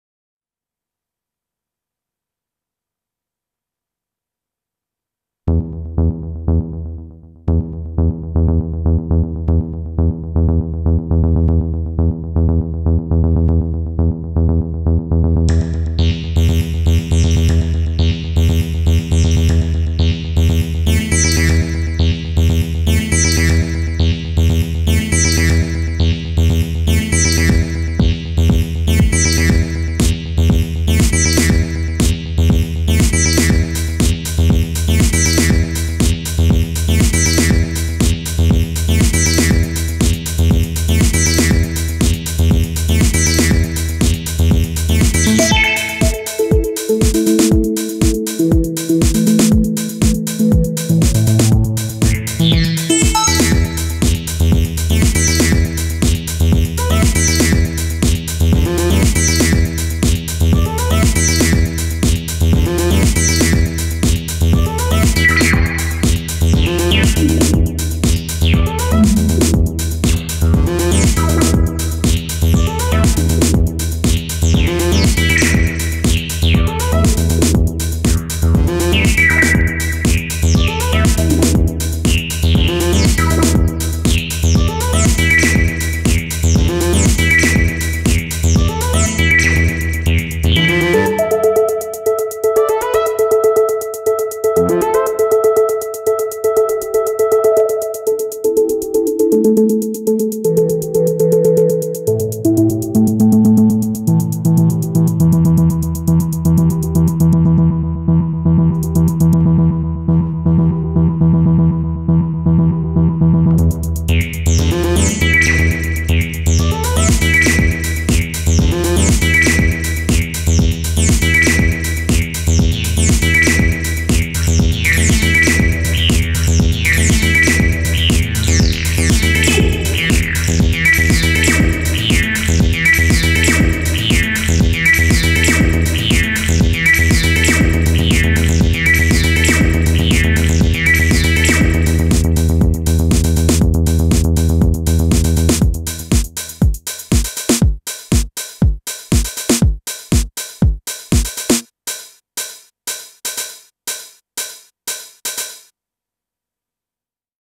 Oscilab- Acid House on an iPad - Luke Neptune.
I'm usually a hardware kind of guy but this little app for the iPad is actually pretty cool.